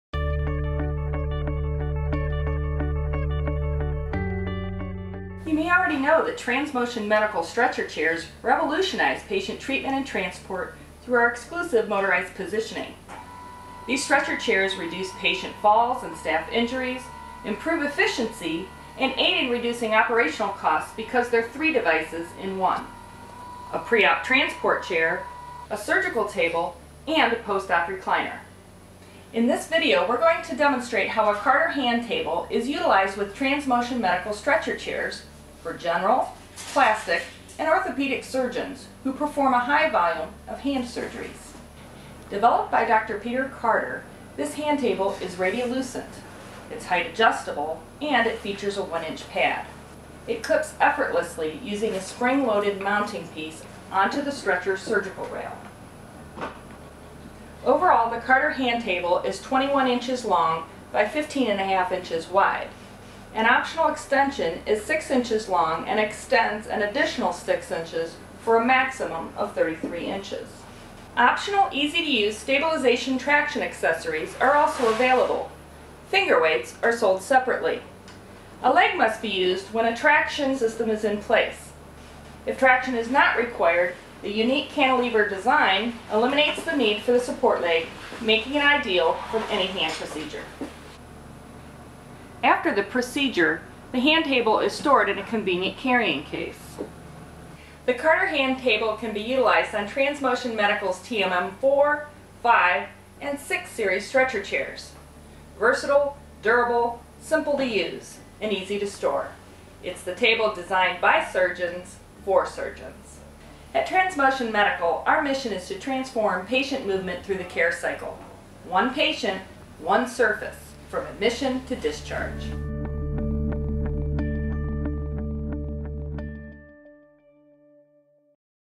Carter Hand Surgery Table
For surgeons who perform a high volume of hand surgeries or for those who just want the best. TransMotion Medical has partnered with Instrument Specialists, Inc (ISI) to offer a new accessory – the Carter Hand Surgery Table. Developed by Dr. Peter Carter, it clips effortlessly on the TransMotion Medical stretcher-chairs surgical rail and allows for easy traction during hand procedures. When traction is not being performed, the unique cantilever design eliminates the need for supporting legs, making this a legless surgical table ideal for any hand procedure.

The Carter Hand Surgery Table is radiolucent, height adjustable, has an optional adjustable-length pad that adapts to individual patient arm lengths, and traction accessories are easily disassembled and conveniently stored in its compact sturdy case. Comes complete with height adjustable leg, 1" foam pad, traction set and storage case.  Versatile, durable, simple to use, and easy to store – it’s the table designed by a surgeon for surgeons. Pictured with the TMM5-XWTB Mobile, Surgical Stretcher-Chair with motorized positioning. (finger weights not pictured)